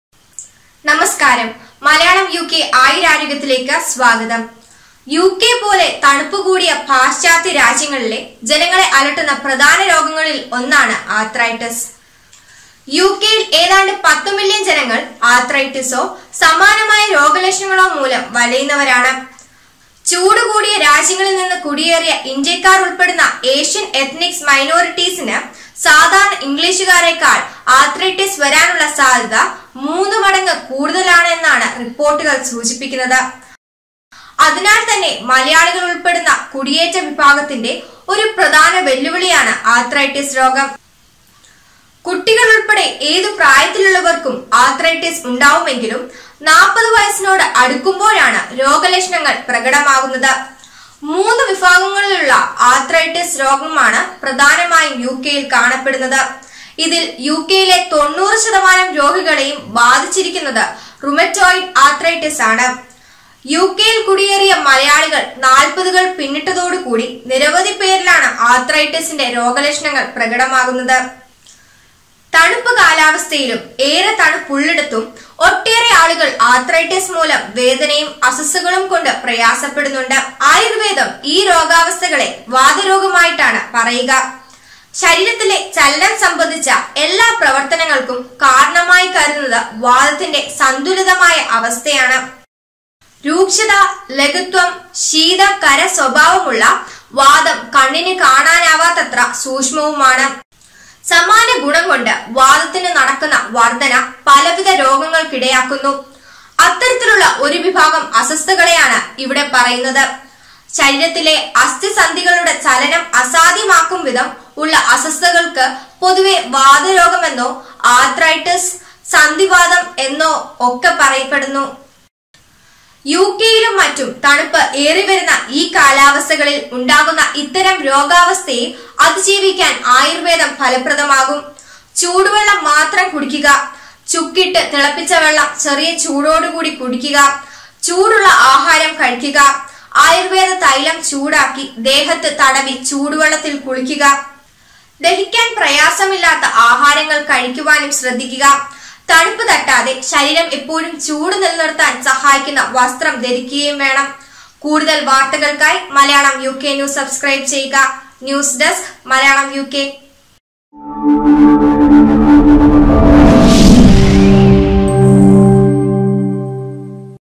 ആർത്രൈറ്റിസിനെക്കുറിച്ച് മലയാളംയുകെ ആയുരാരോഗ്യത്തിൽ ഡോക്ടർ എ. സി. രാജീവ്‌ കുമാർ
പാശ്ചാത്യലോകത്ത് മലയാളികളുടെ പ്രധാന ആരോഗ്യപ്രശ്നങ്ങളിലൊന്നായ ആർത്രൈറ്റിസിനെക്കുറിച്ച് ആയുരാരോഗ്യത്തിൽ ഡോക്ടർ എ. സി. രാജീവ്‌ കുമാർ